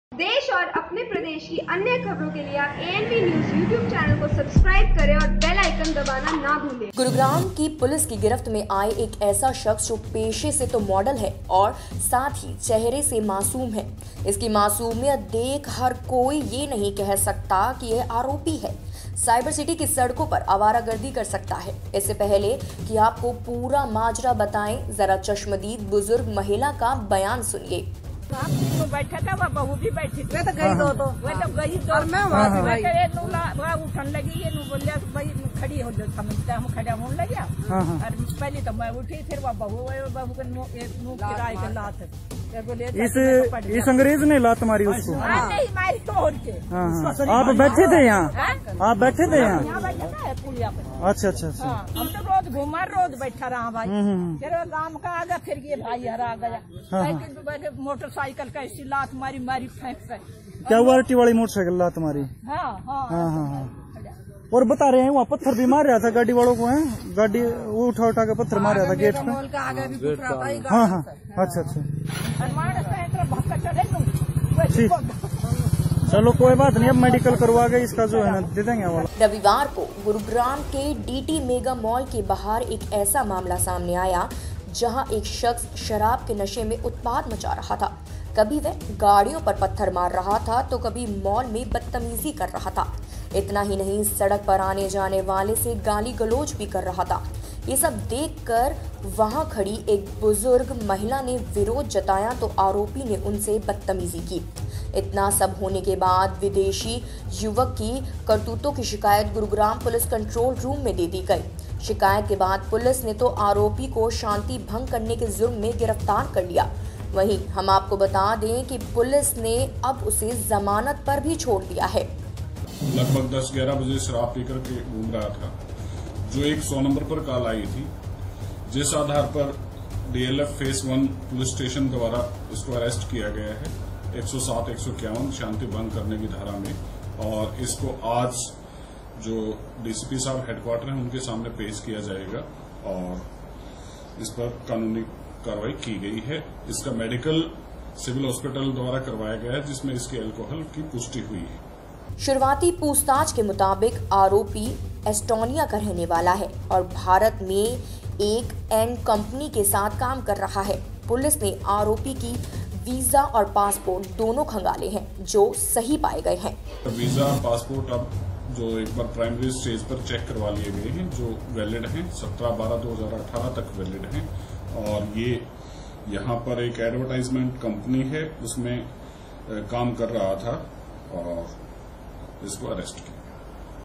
ANV NEWS || नशे में धूत विदेशी मॉडल का हंगामा
रुग्राम पुलिस के गिरफ्त में आए इस शख्स का नाम बुलादी स्लॉय हैं ..ये शख्स पेशे से मॉडल है..चेहरे पर मासूमियत का आवरण लपेटे हुए इस शख्स को देख कर कोई ये नहीं कह सकता की ये आरोपी साइबरसिटी की सड़कों पर आवारागर्दी कर रहा होगा. इससे पहले की आपको पूरा माजरा बताएं जरा चश्मदीद रही इस बूजुर्ग महिला का बयान सुनिए.